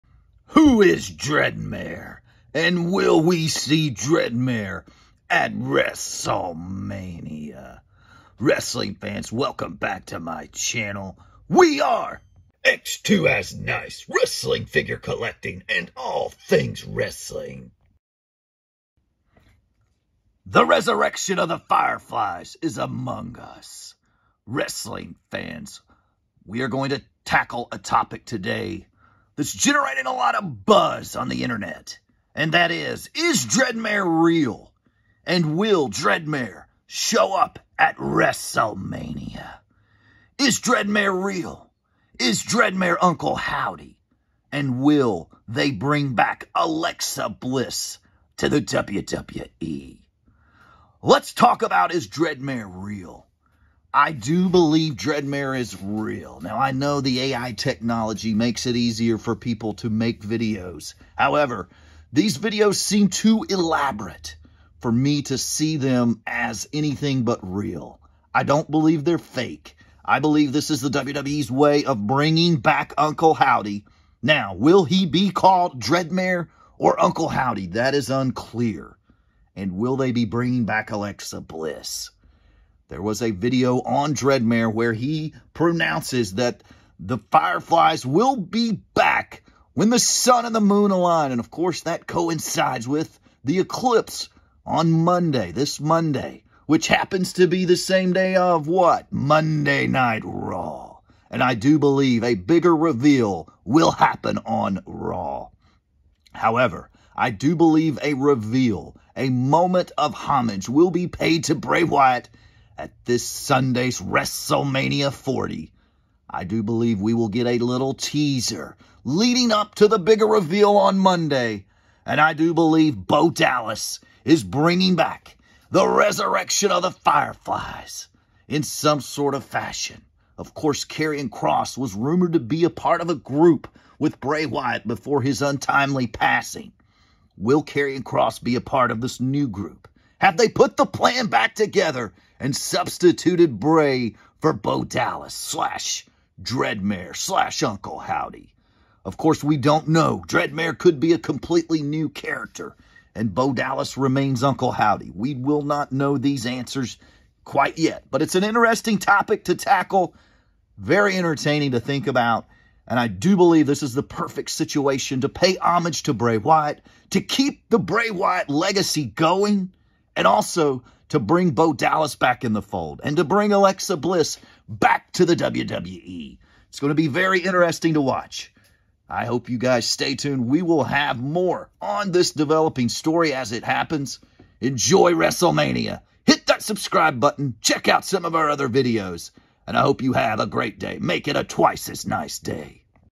Will Dreadmare be at WrestleMania?
Wrestling fans, WrestleMania is here! We are just hours away from Day 1 of WrestleMania XL 40! There’s been this thought weighing in the back of my mind for the lead up to Mania! Is Dreadmare real? Will Dreadmare be at WrestleMania XL?